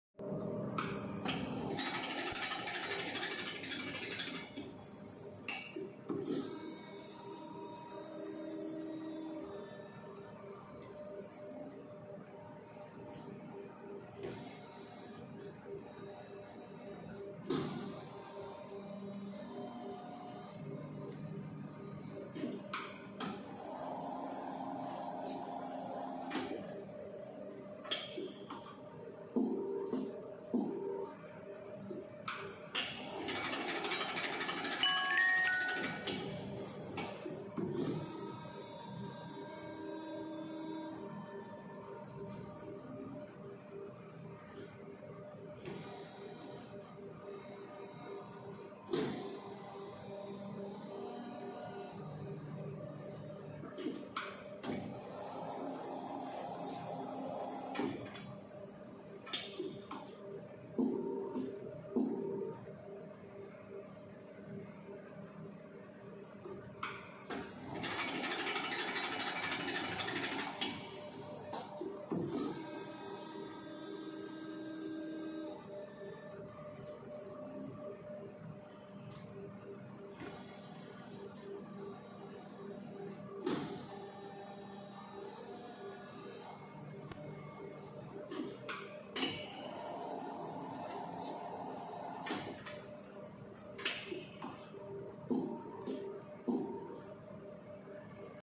Taiwan Kinki 120t used Vertical Plastic Injection Molding Machine
Machine can load 1 upper mold and 2 lower molds to work at the same time, which increases production output. It is in good working condition.